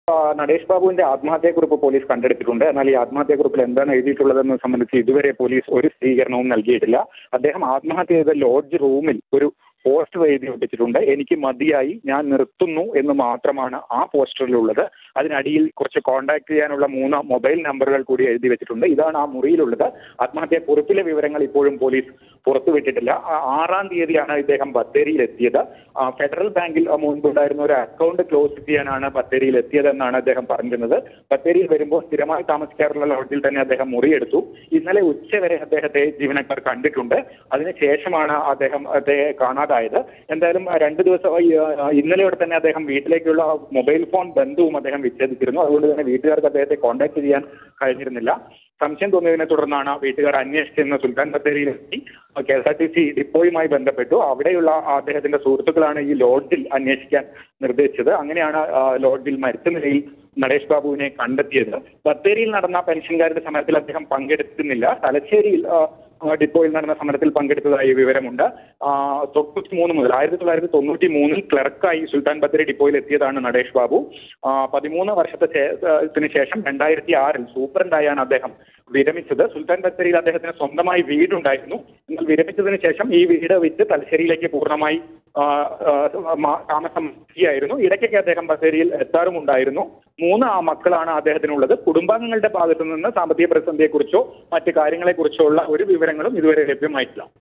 Former KSRTC Supdt Kills Self In Wayanad| Mathrubhumi News
Wayanad: Former KSRTC employee Natesh Babu committed suicide reportedly due to financial crisis as he was not getting pension for the past few months. He was formerly superintendent at the Bathery depot of the KSRTC. The police have recovered his suicide note, but they haven't divulged its contents. On the wall of his lodge room was found pasted a poster saying "I have had enough, I am ending it."